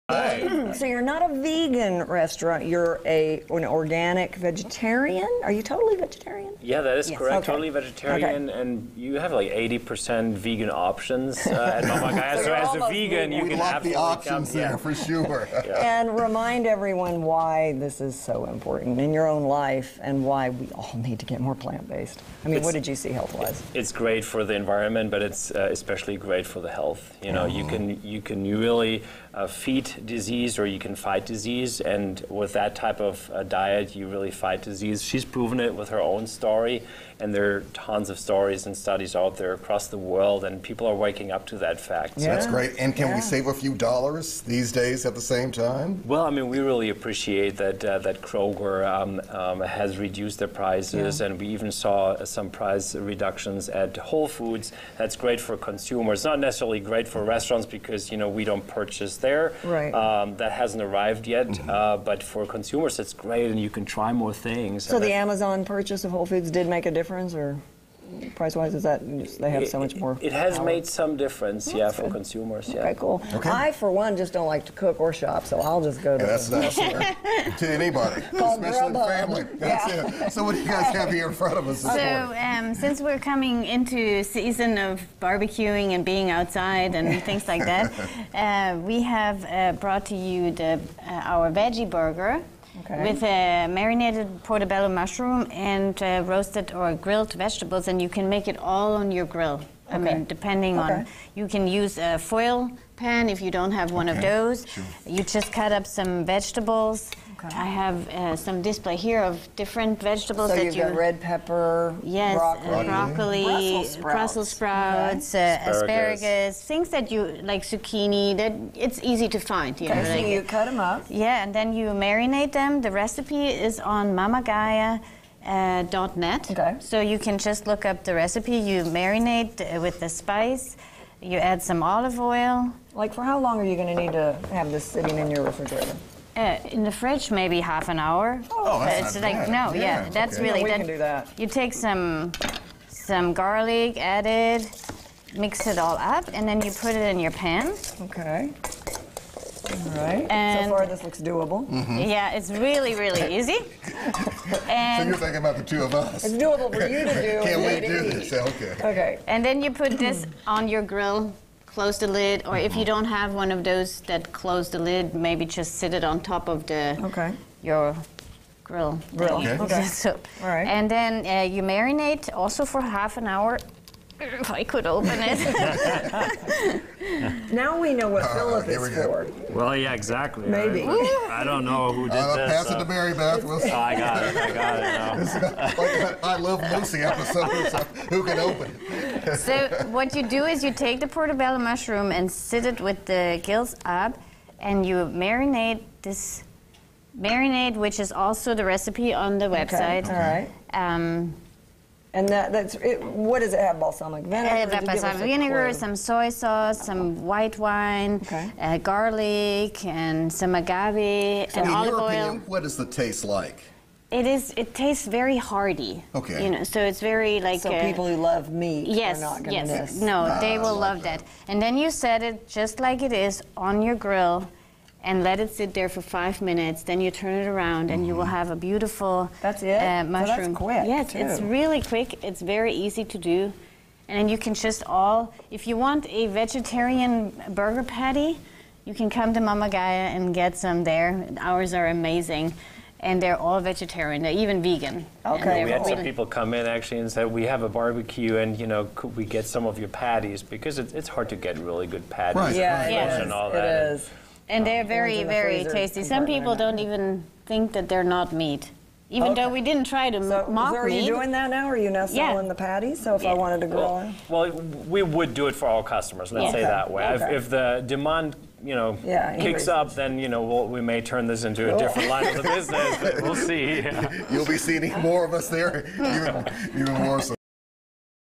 Cooking with Mama Gaia
With a little time and a corner of your backyard, you can save a lot of money on groceries and eat healthier at the same time. For those who don`t want to grow their own vegetables or shop organic, let Philipp and Cru Von Holtzendorff-Fehling do the cooking for you at Mama Gaia in Midtown.